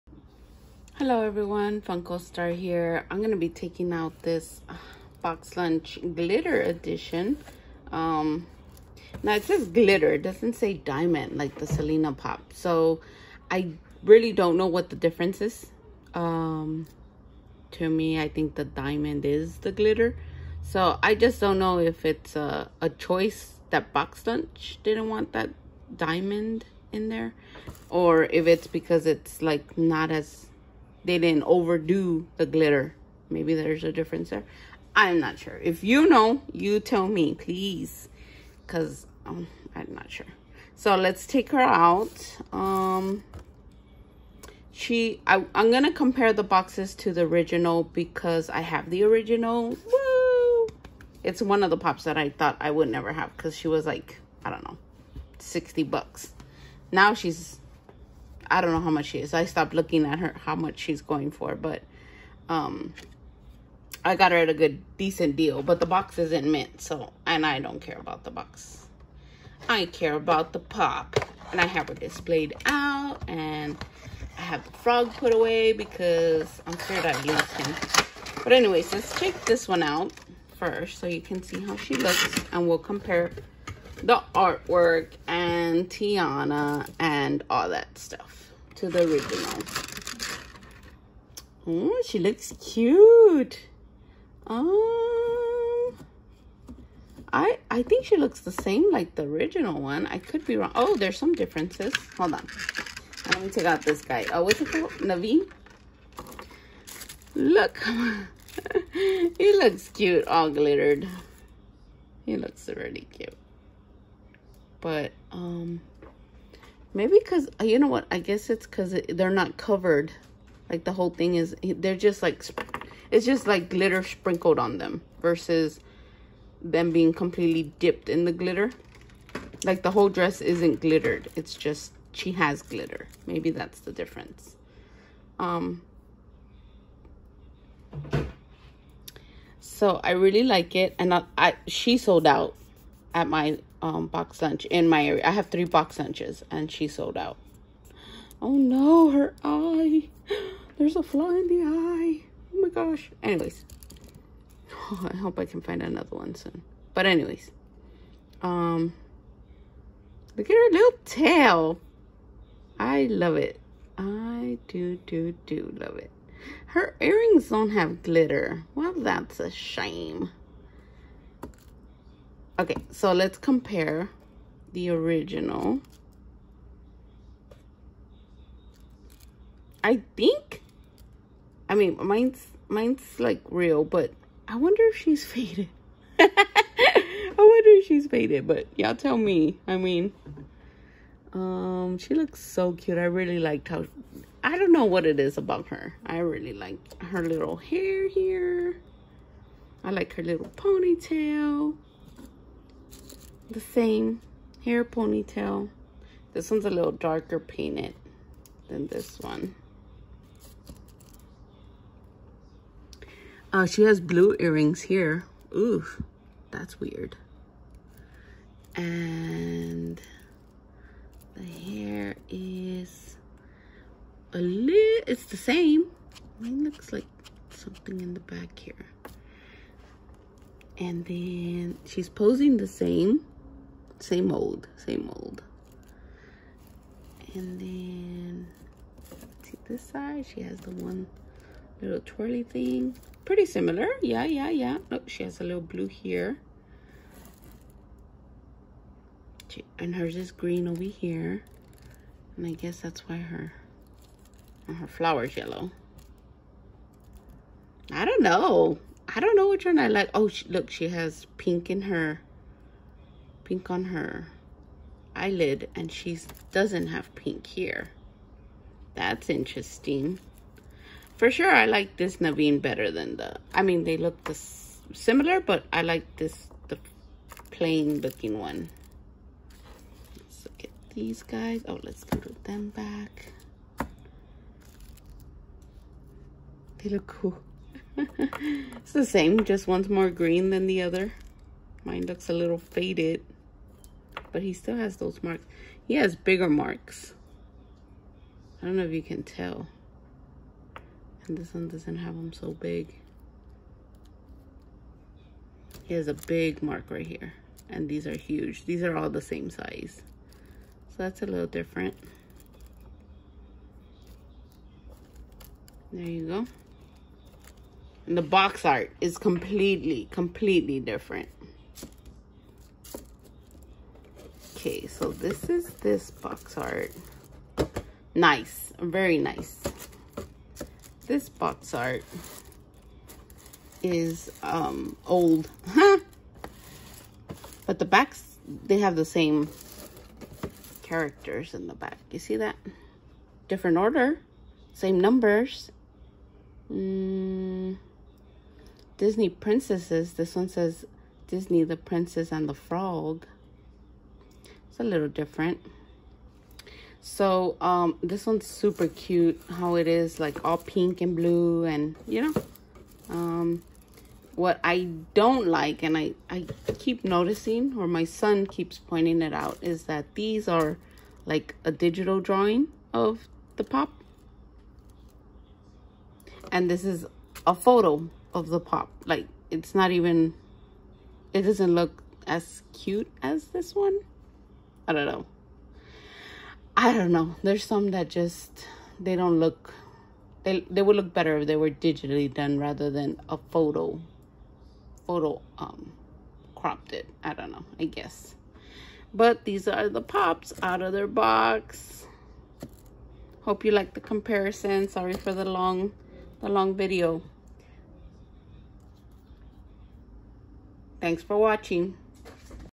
Boxlunch glitter exclusive Tiana n Naveen
In this video I compare the original Tiana pop w/the new Boxlunch exclusive .